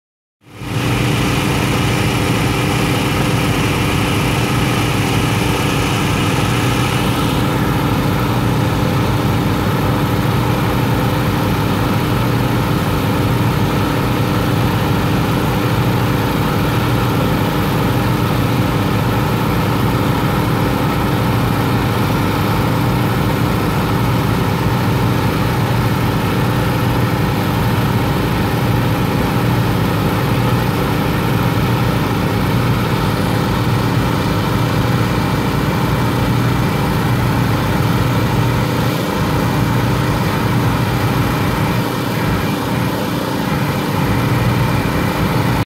The AQUALOGIX USA system takes muck sucking literal.
AQUALOGIX USA, LLC  The logical solution for pond cleaning and micro-dredging.  Here is how we literally suck the muck! Our methodology allows us to go where the big boy's can't, we can also be very target specific or just do the whole pond, eliminating the muck, sediment and debris!